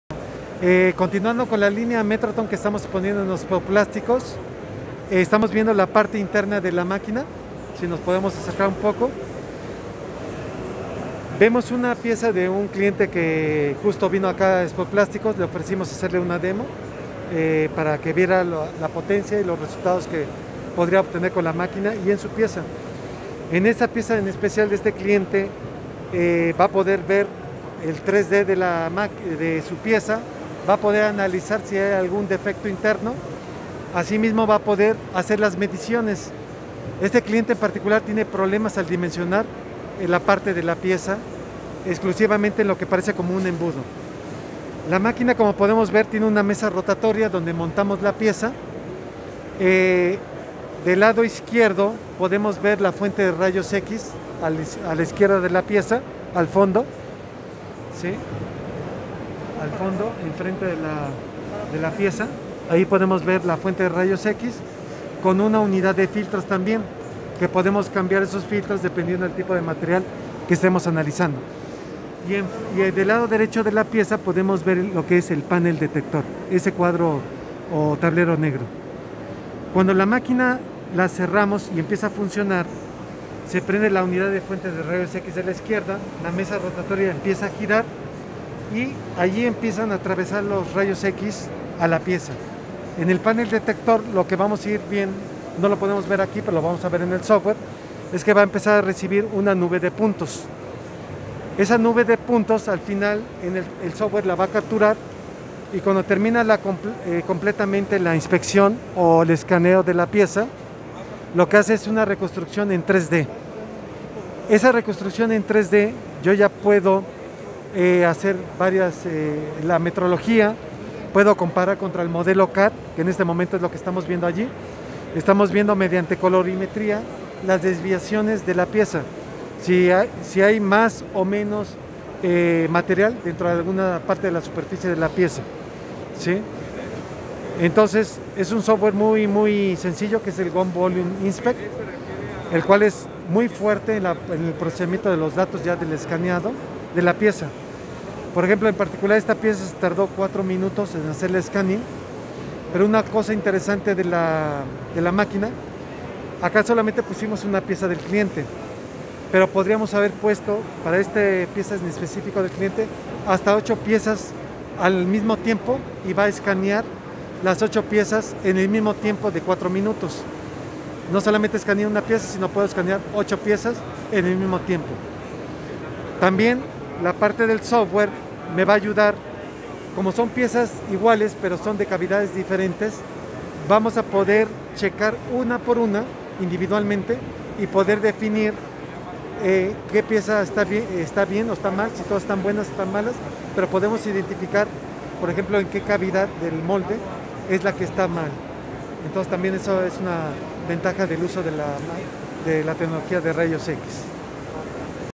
Zeiss | Reconstrucción 3D y análisis por colorimetría | Expo Plásticos 2021
¡Resultados demostrados en vivo desde Expo Plásticos 2021! Soluciones de metrología de Zeiss mediante reconstrucción 3D y análisis por colorimetría. En tan solo 4 minutos se puede reconstruir una pieza para validar sus dimensiones.